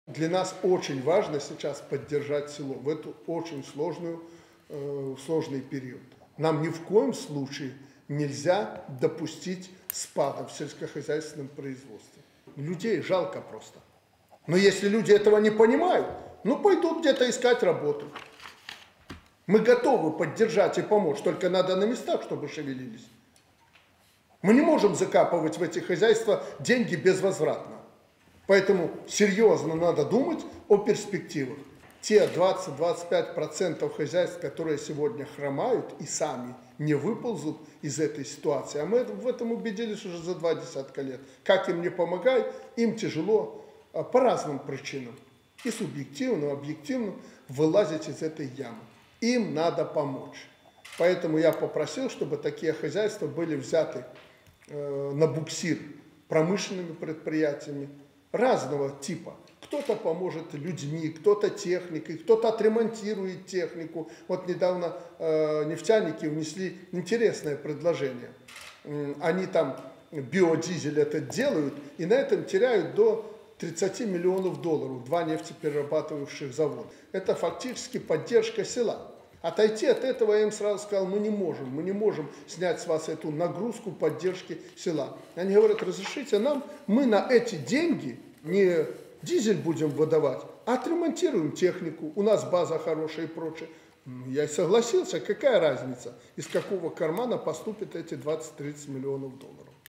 "Просто жалко людей" - Лукашенко предложил промпредприятиям взять на буксир слабые хозяйства
Президент Беларуси Александр Лукашенко выступает за поддержку отстающих хозяйств силами промышленных предприятий. Об этом он заявил сегодня, согласовывая назначение руководителей местных органов власти.